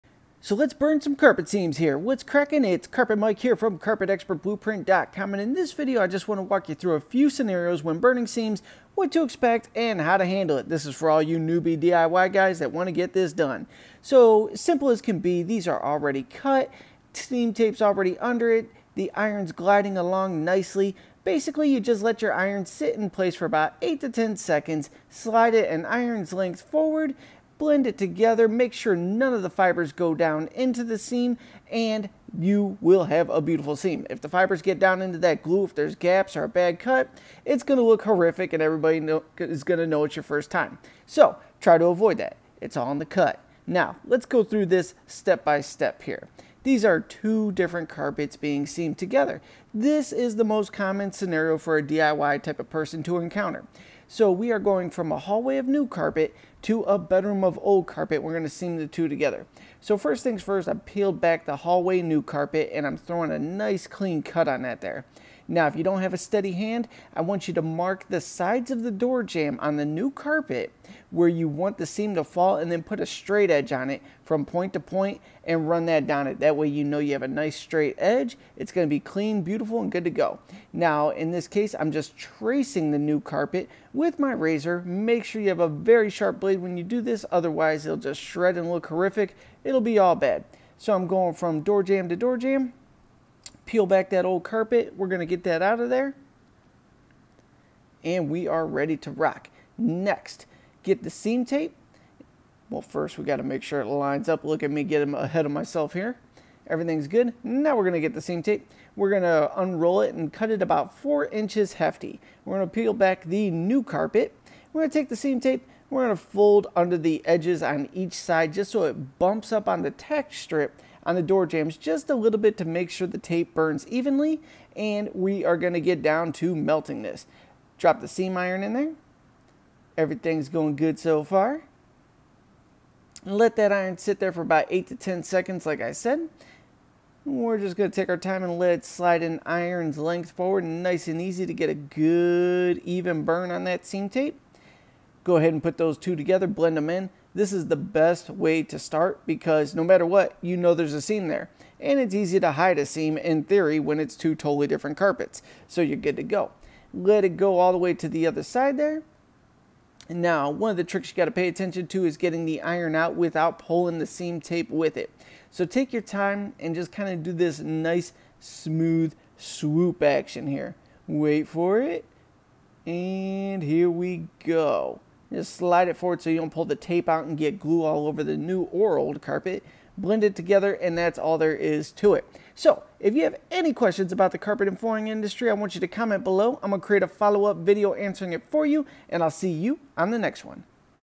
🔥 Carpet Seams 🔥TIPS ON TIPS ON TIPS
In this video I share a few tips on how the diy world can tackle carpet seams successfully!

All the carpet goodness and profit strategies can be found